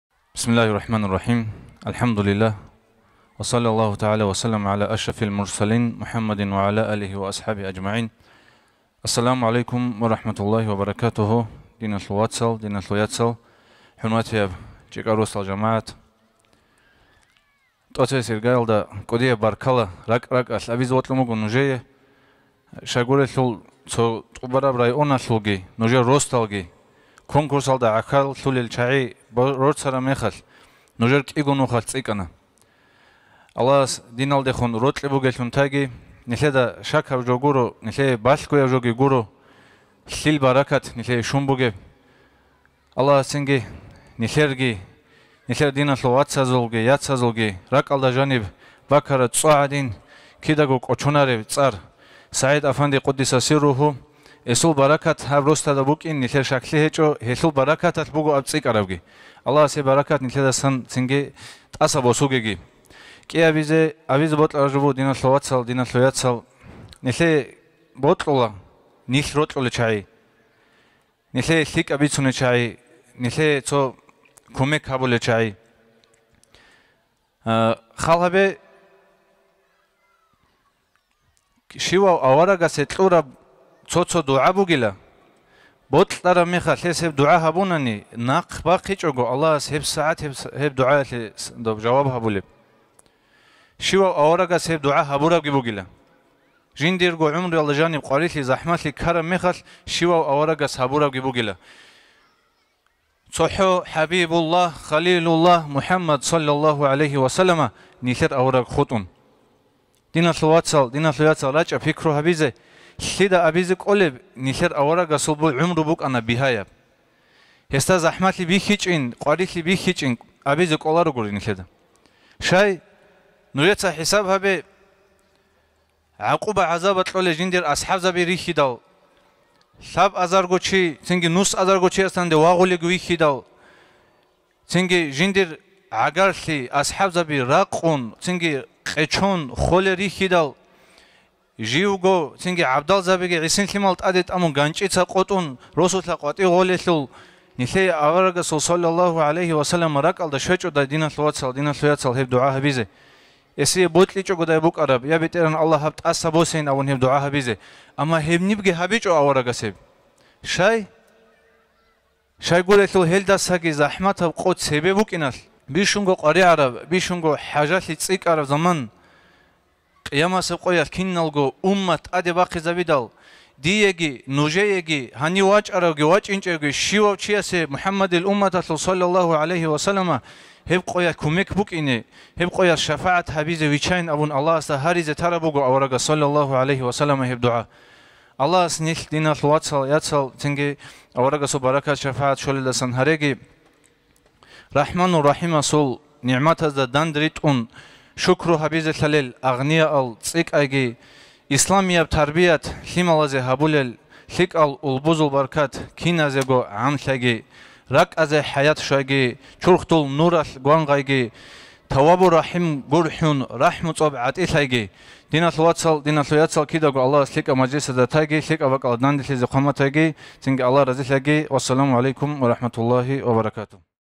Выступление имама г. Буйнакск и Буйнакского района Алиева Мухаммада на маджлисе вручение призов
1 этапа конкурса "Благонравие праведников" в с. Чиркей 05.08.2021 г.